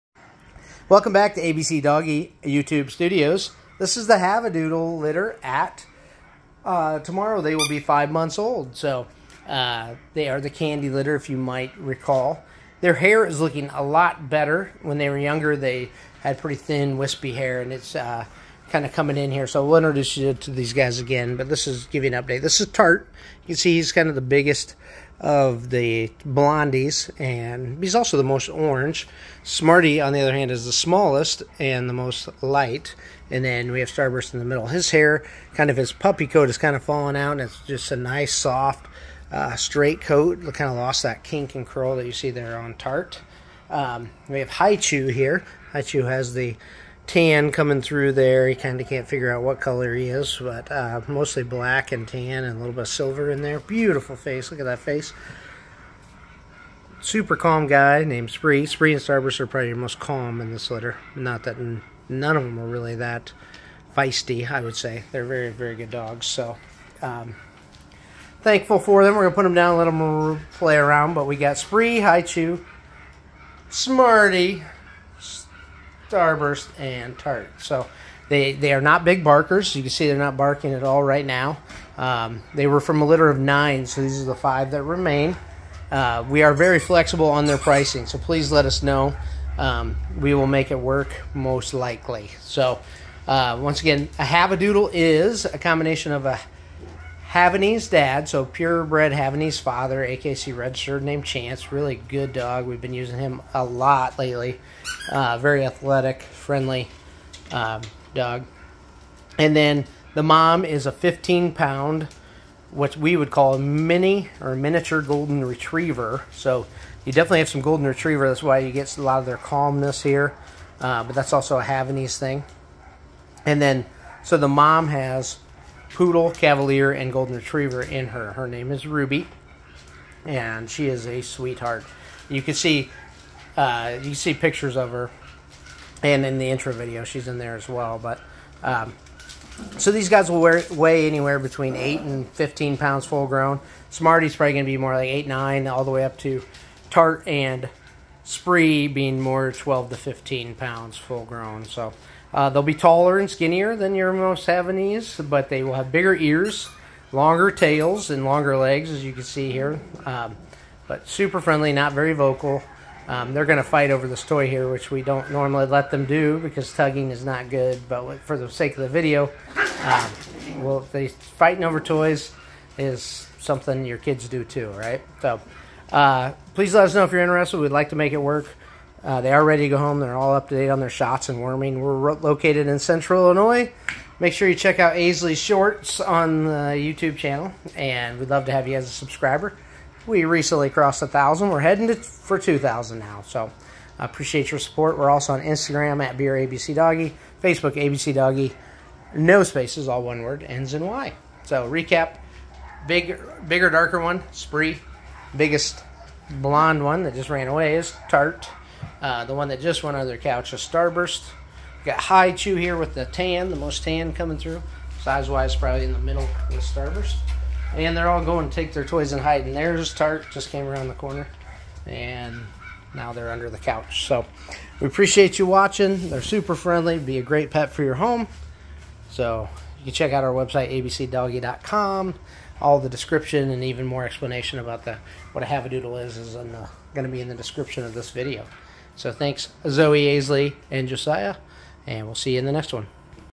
Havadoodles * Candy Litter * 5 months old * ABCdoggy 2023
Havadoodles are the offspring of a 100% AKC Registered Purebred male (Chance) & a Miniature Golden Retriever (Ruby).  They will resemble Havanese the most, but will have bigger ears and longer legs.  Mostly non-shedding and very low allergy response as well.

15+ Years experience, Health Guarantee, Roanoke Illinois

Facbook: abcdoggy

Our goal is 2000 by the end of 2023!